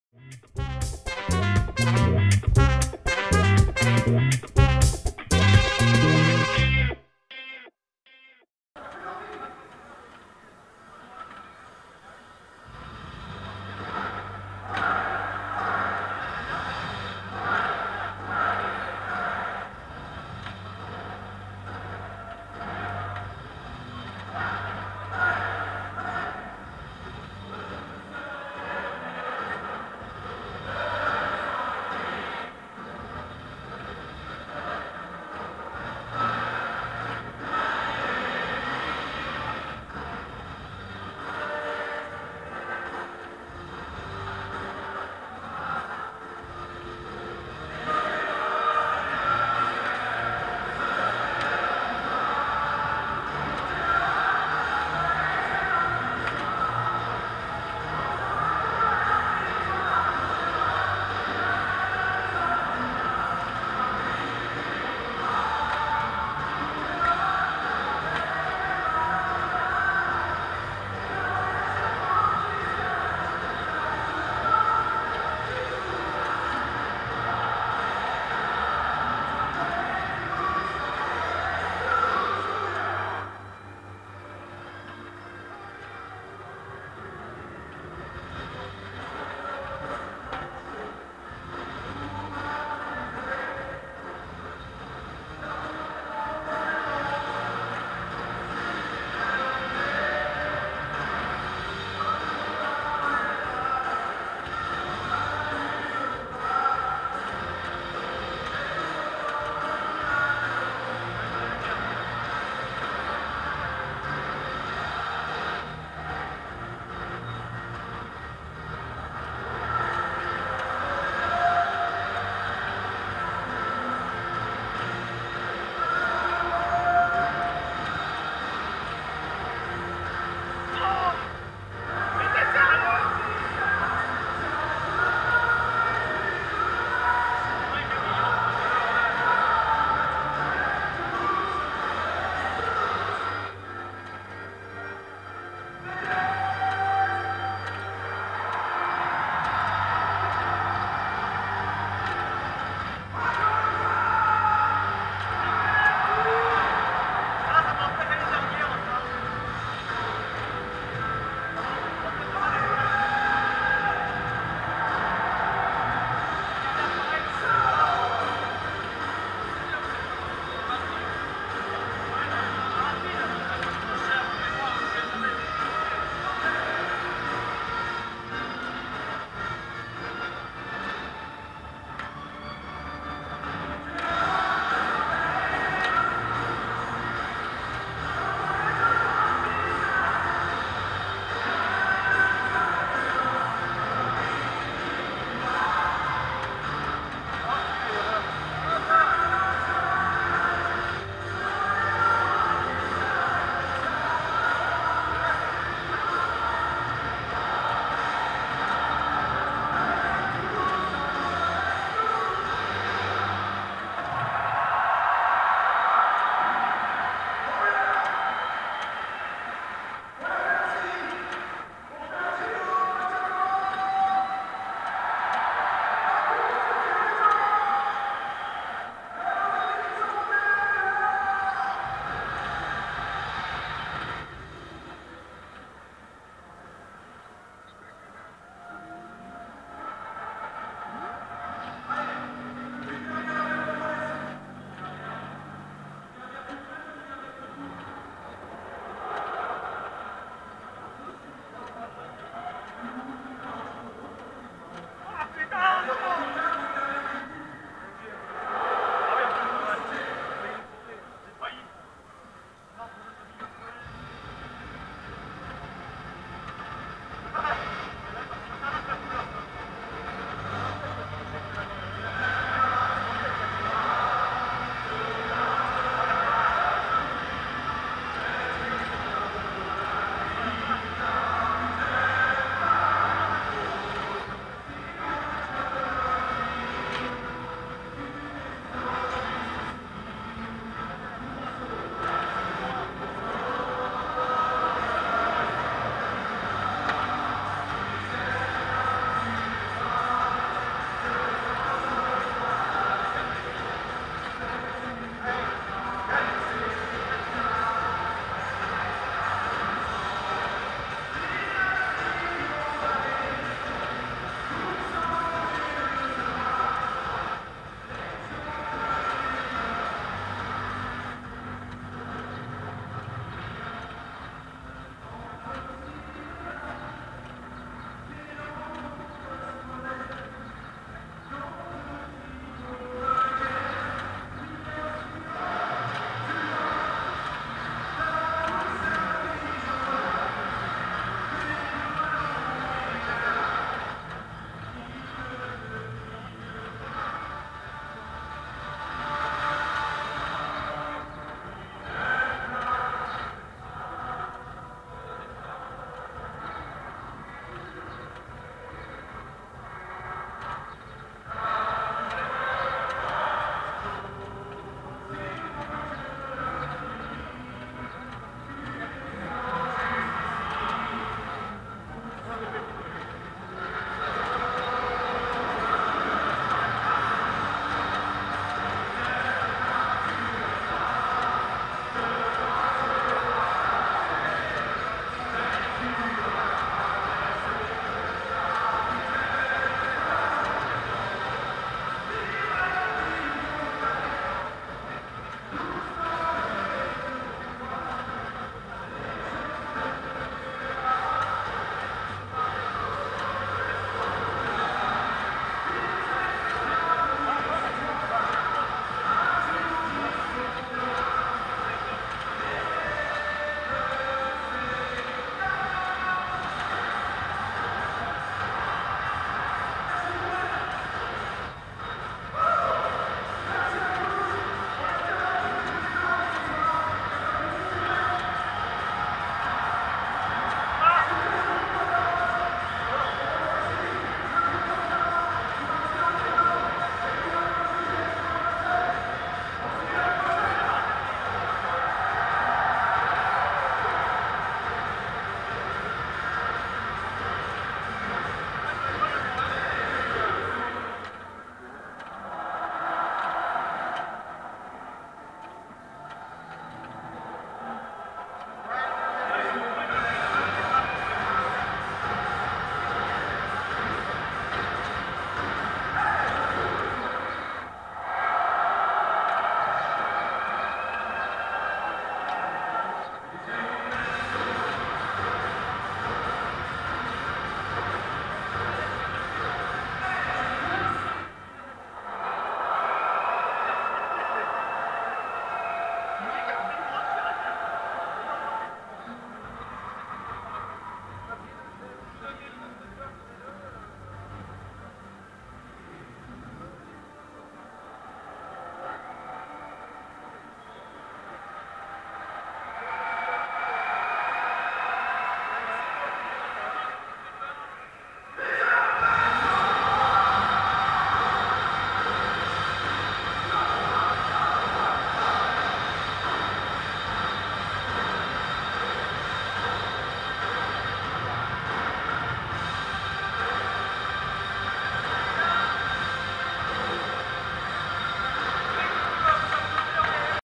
Finale Tournoi Samedi TGS 2016
Finale du tournoi organisé par Toulouse Multiball à l'occasion du Toulouse Game Show le samedi 26 novembre 2016.